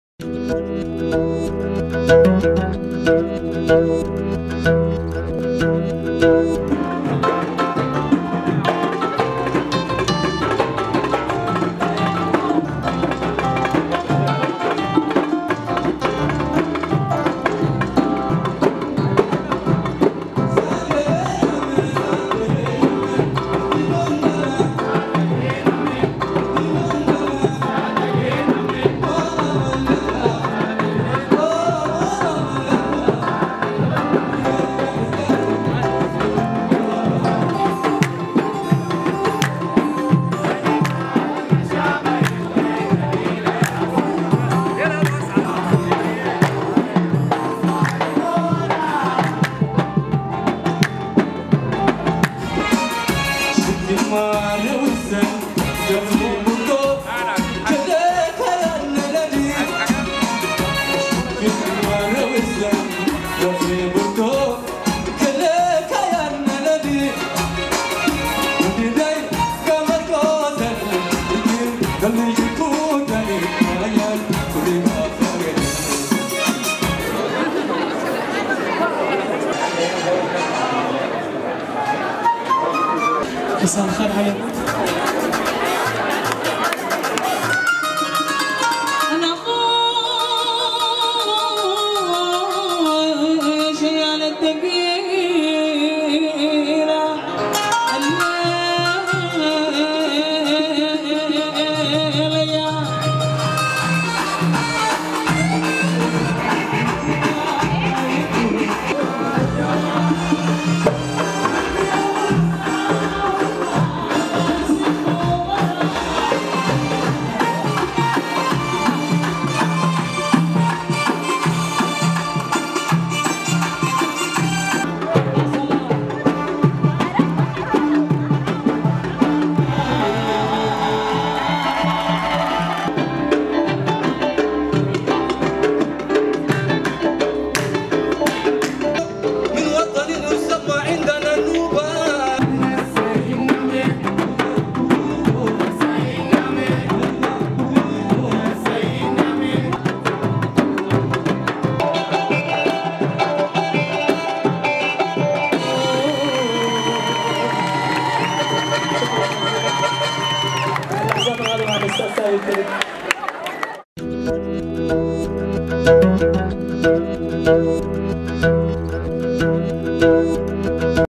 حفل نوبي سوداني في بيت السناري
حفل في حب النوبة حيث تقيم فرقة بوهين النوبية ليلة غنائية استعراضية نوبية وسودانية مع الفنان المطرب ثابت تبد، والفنانة السودانية ريم الثورة.